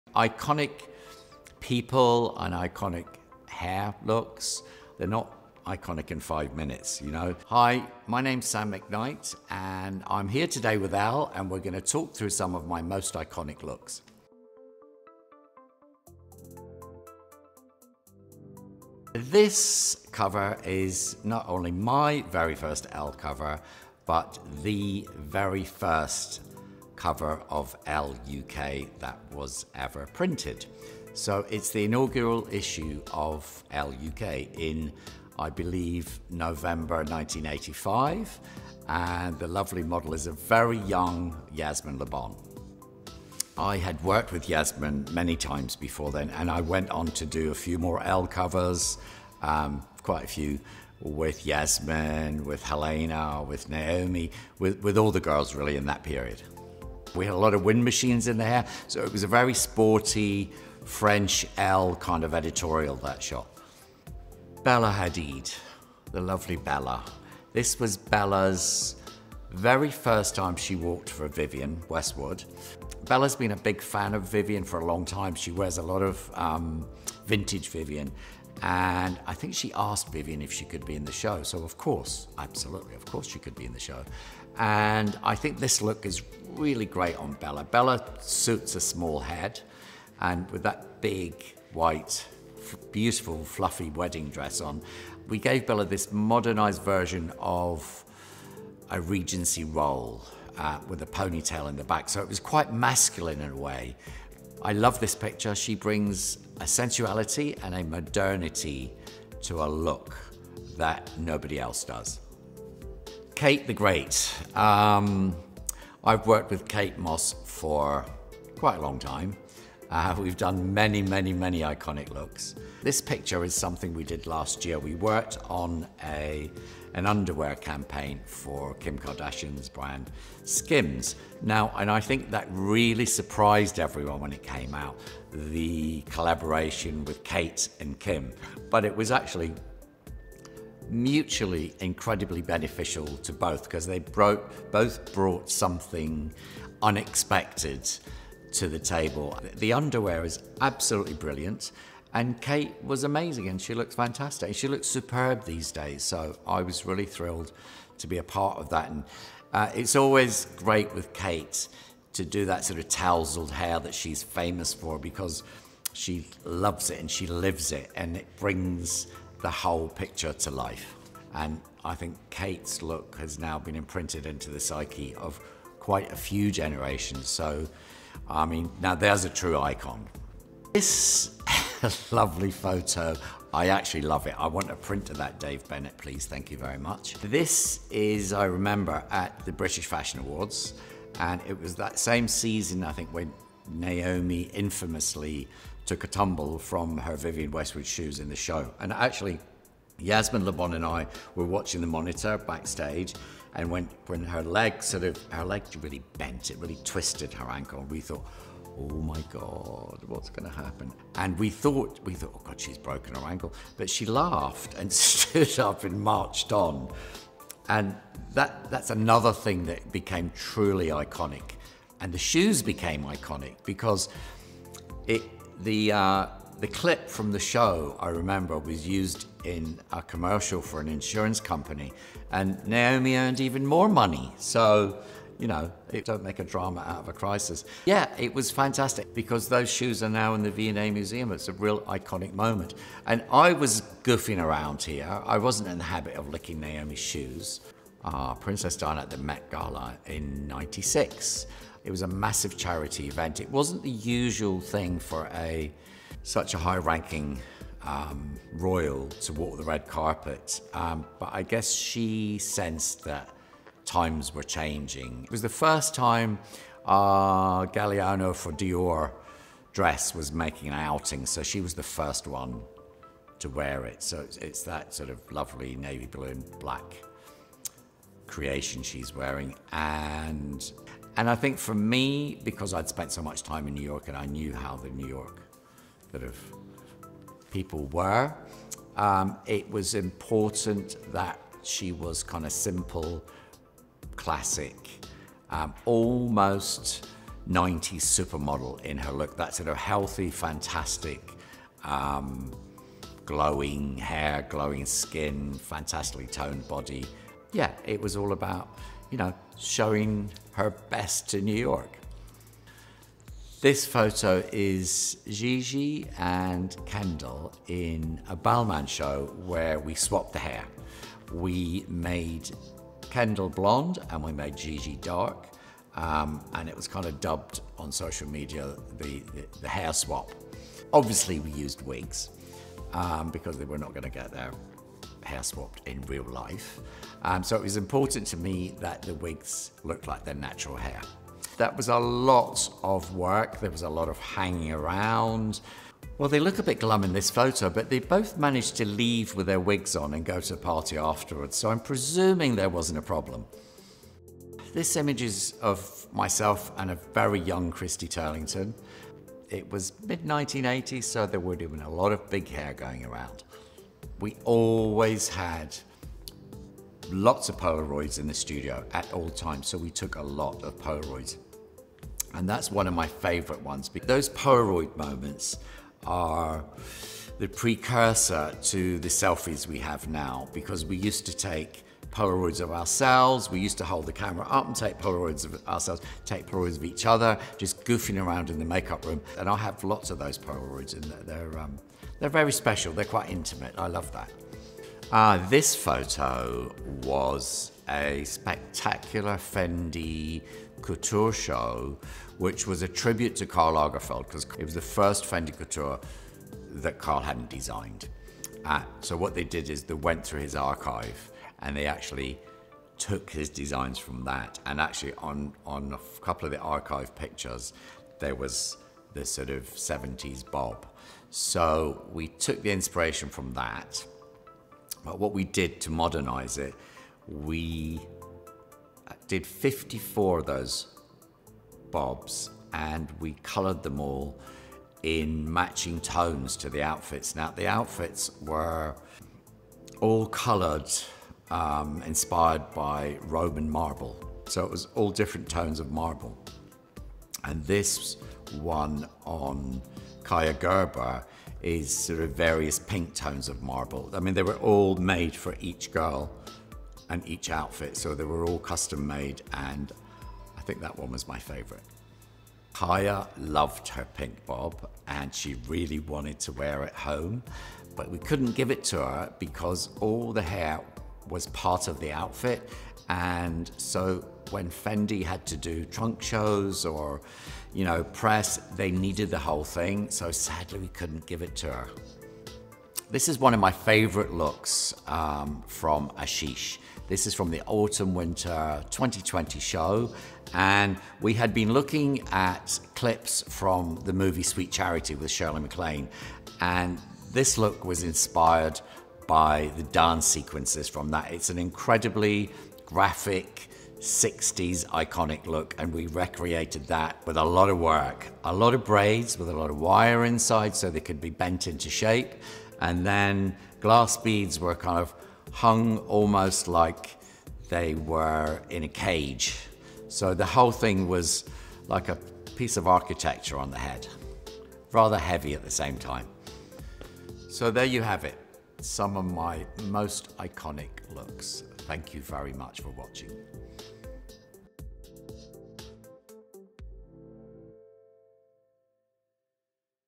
Legendary Stylist Sam McKnight Walks Us Through Some Of His Most Iconic Hair Looks Of All Time
Llegendary hairstylist Sam McKnight delves into the archives to share his most iconic hair creations for ELLE UK's latest video series, It's Iconic.